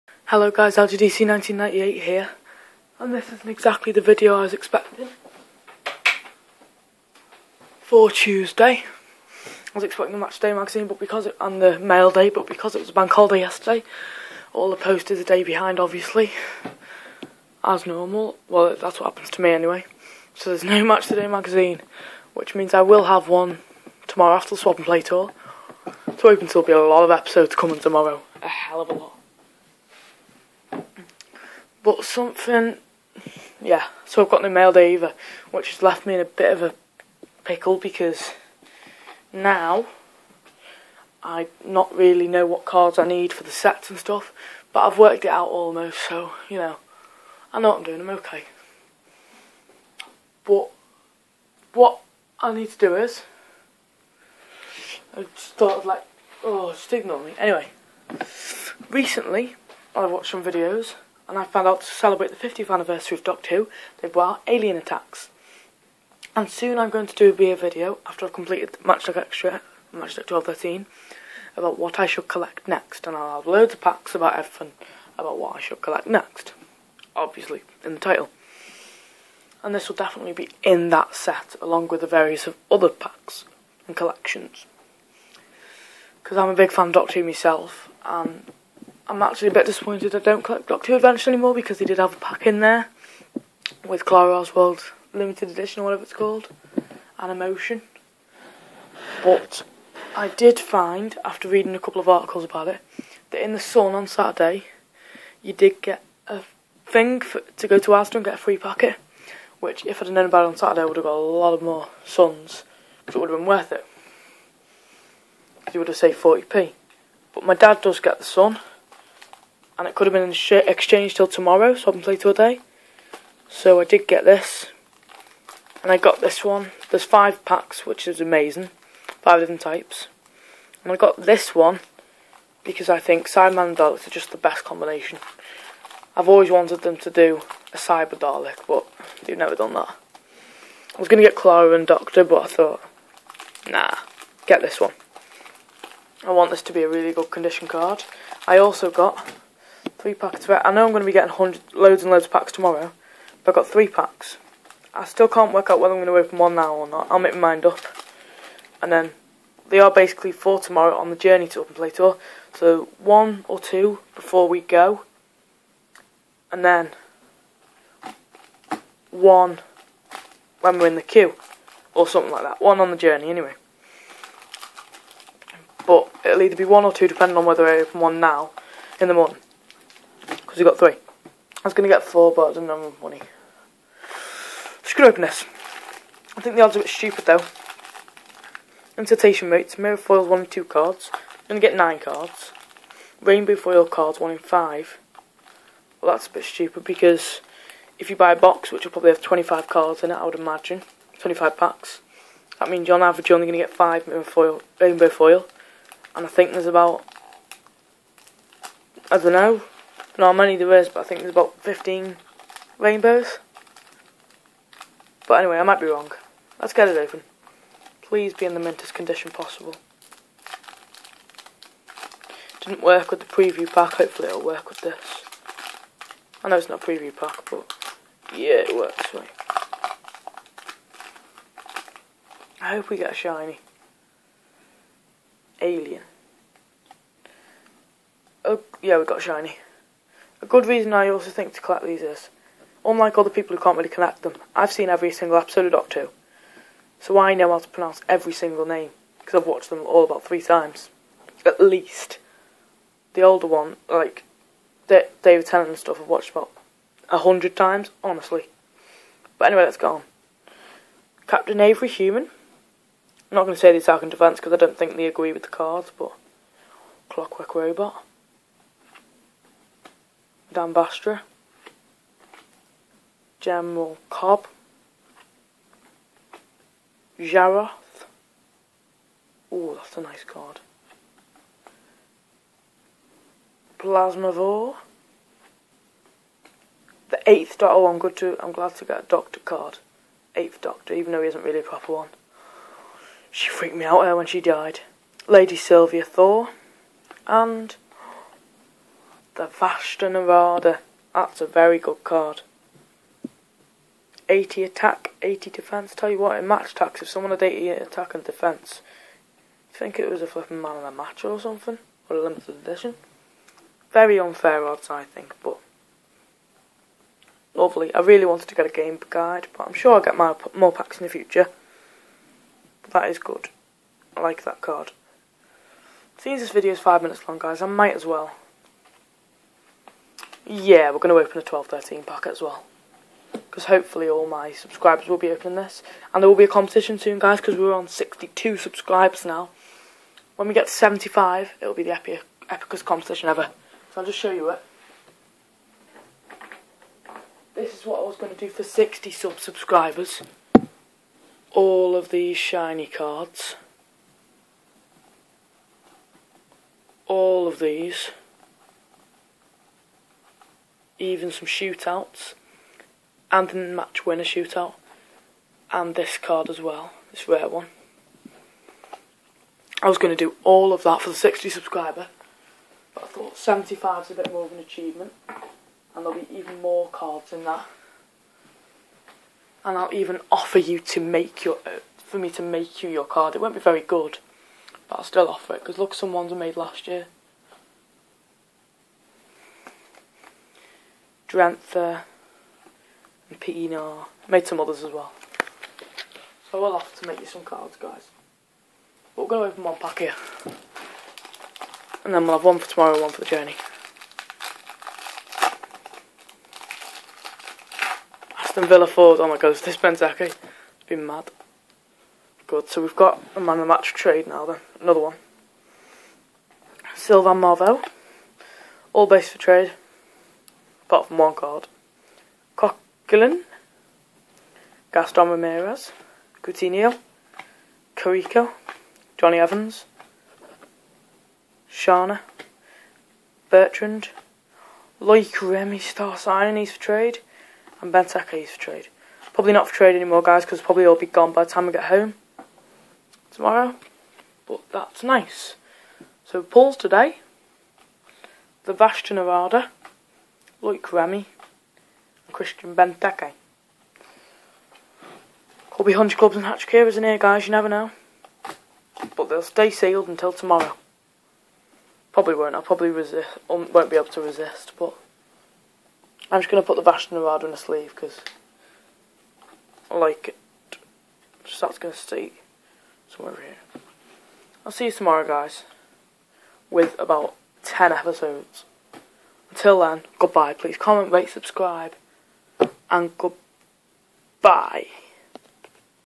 Alien attax pack and match attax extra pack!!
Hey guys today we have one pack of alien attax, could these be my new cards to collect? Who knows,and we also open a pack of match attax extra before our epic swap and play tour episode tomorrow!